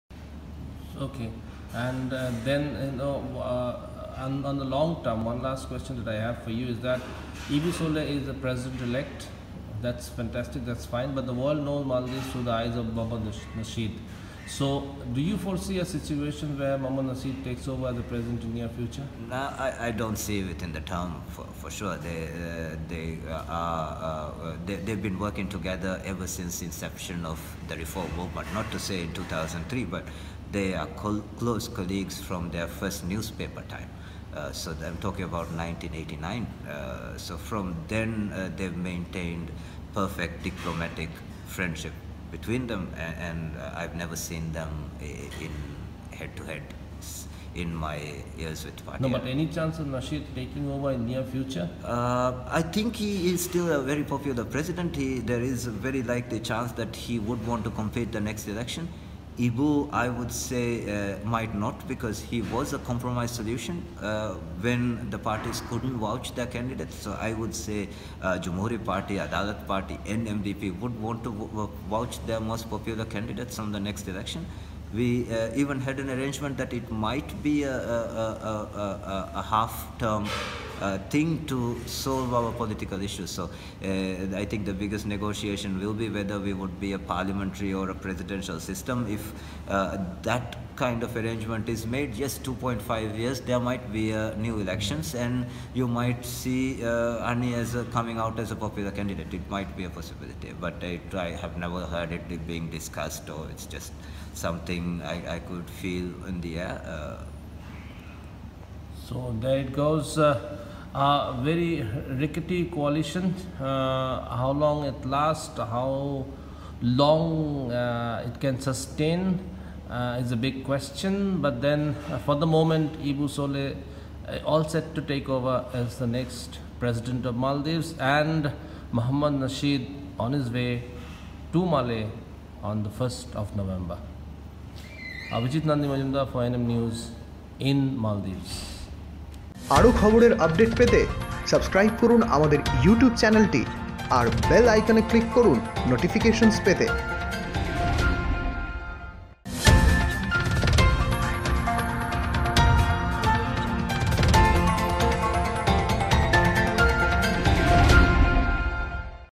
আগামী নির্বাচনে রাষ্ট্রপতি পদ প্রার্থী হতে পারেন মহম্মদ নাশিদ
অভিজিৎ নন্দী মজুমদার, এডিটর ইন চিফ, মালদ্বীপ: স্বদেশকে কর্তৃত্ববাদের কবল থেকে রক্ষা করতে দক্ষিণ এশিয়ার বৃহৎ শক্তি ভারতের শরণাপন্ন হয়েছিলেন  মহম্মদ নাশিদ। প্রকাশ্য বিবৃতি দিয়ে ভারতের হস্তক্ষেপ চেয়েছিলেন মালদ্বীপের ক্ষমতাচ্যুত প্রেসিডেন্ট মোহাম্মদ নাশিদ। কিন্তু মালদ্বীপের মানুষ এখনই মহম্মদ নাশিদের পুনরায় ক্ষমতায় ফিরে আসার কোন সম্ভাবনা দেখছেন না।